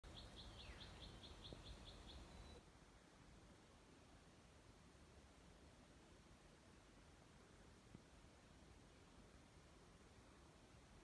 ❇️Snake in the backyard, who knows their snakes ?!
Who knows their snakes? :)